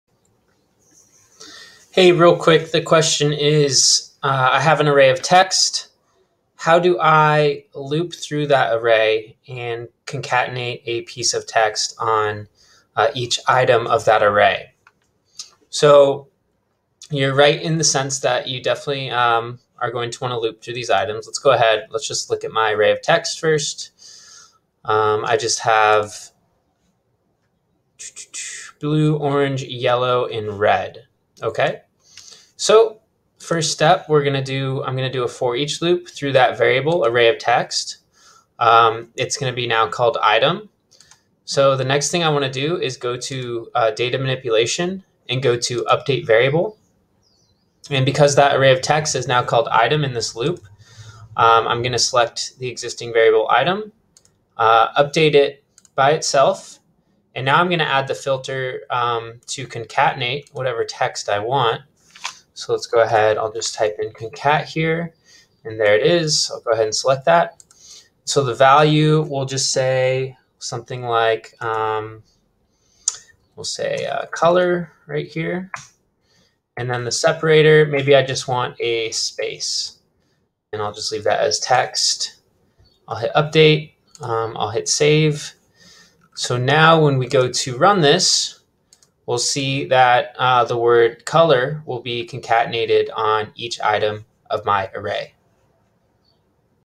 How to loop through an array and concatenate text to each item
Customer question: How do I loop through an array of text and concatenate more text to it (manipulate)?

Here's a snippet that provides the function demonstrated in this video

Vist Xano - The Fastest No Code Backend Dev Platform